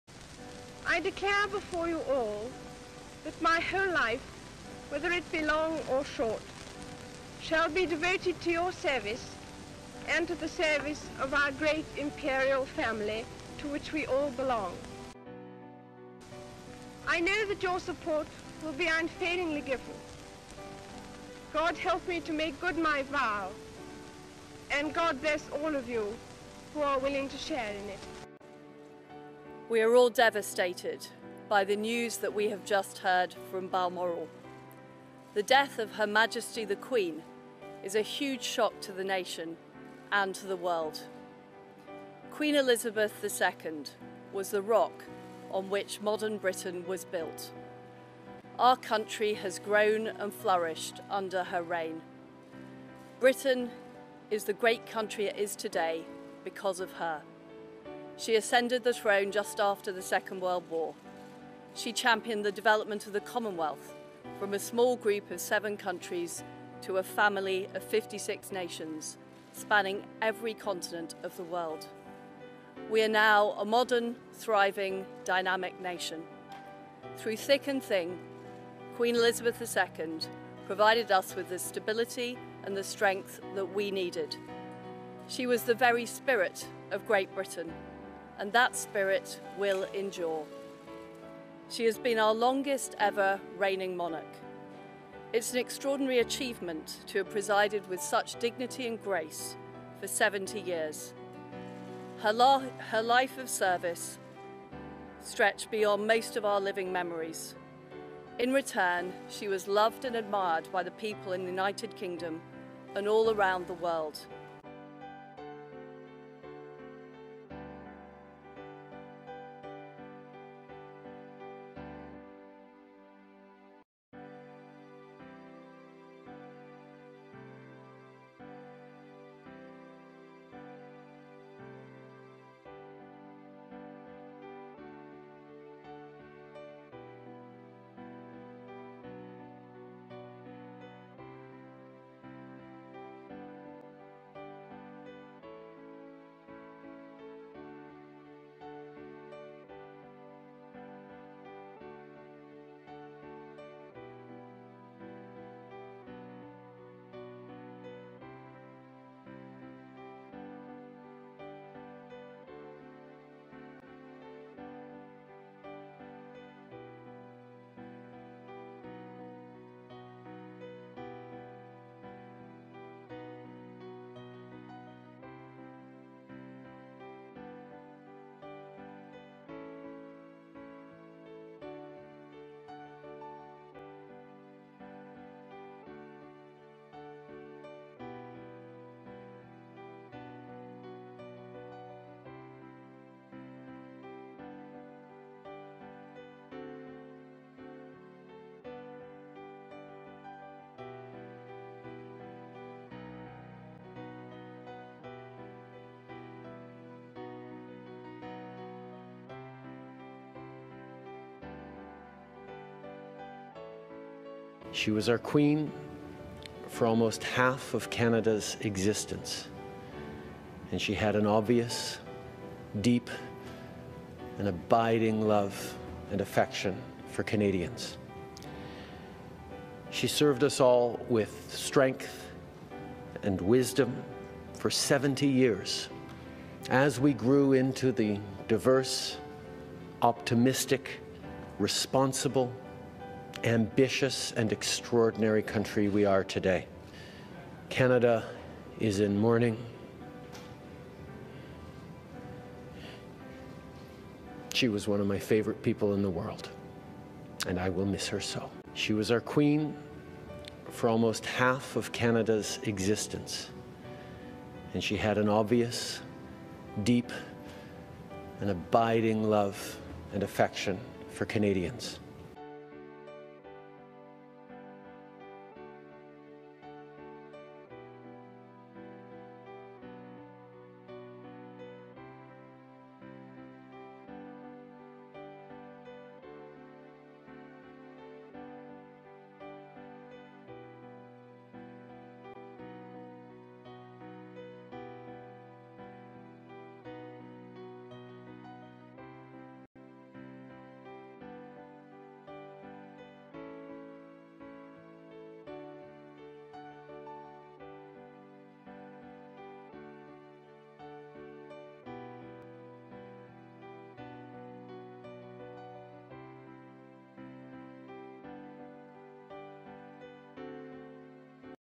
Tributes to Queen Elizabeth
At the age 96 and after reigning for 70 years, Queen Elizabeth II has died peacefully at her Balmoral Castle home. Tributes have poured in from all over the world for this one-of-a-kind lady who was queen when Churchill was Prime Minister! World leaders, Presidents and Prime Ministers are among those who have released heartfelt statements, see what only a few of them have said, Including Joe Bidden, Macron, Liz Truss, Trudeau. Also hear in her own words what the queen said before embarking on her journey to be monarch.